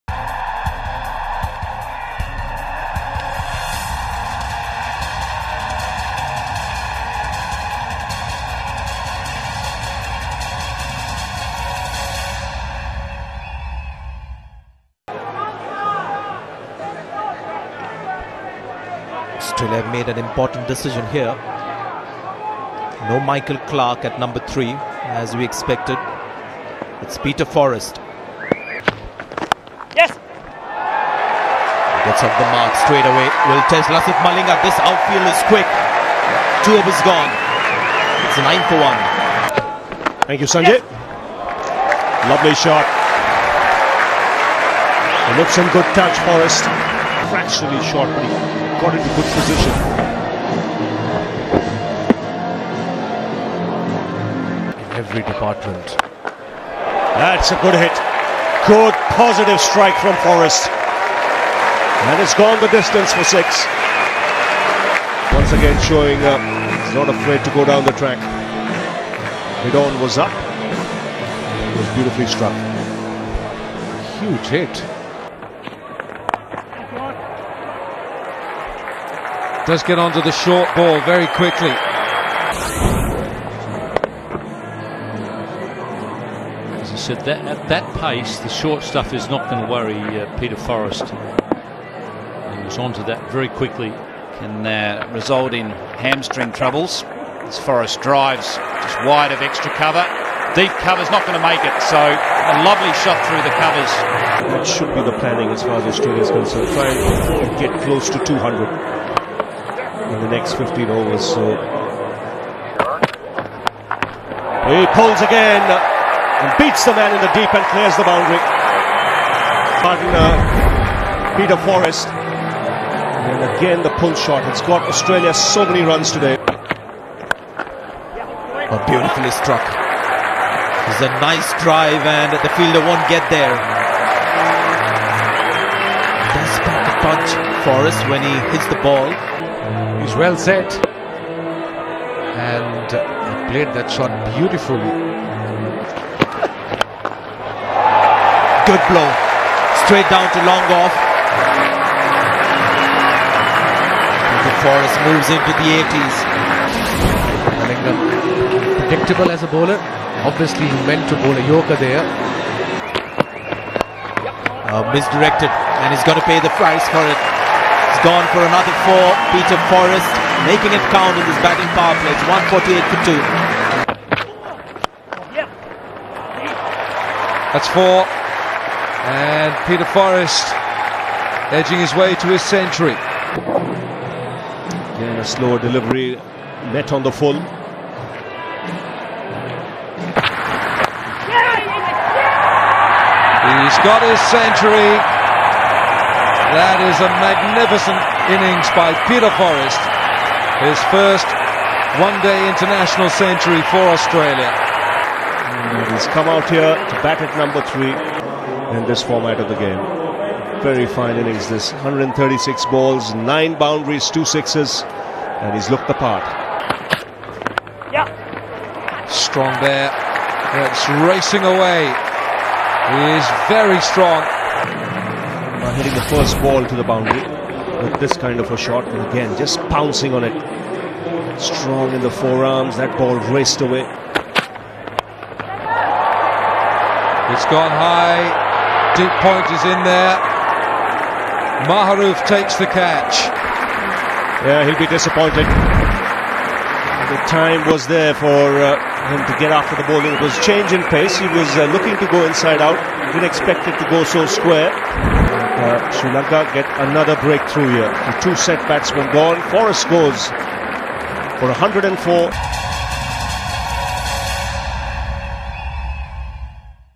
CB Series: Australia vs Sri Lanka, Peter Forrest- 104 runs
Peter Forrest scored an impressive hundred against Sri Lanka in the 9th match of the on-going CB series at Bellerive Oval in Hobart. After scoring a promising 66 on debut against India, Forrest scored his maiden ODI hundred in his 4th match. The 26-year-old replaced Ricky Ponting at number 3 in the Australian ODI squad. The Queenslander is the first Australian batsman to have three fifty plus scores after just four games.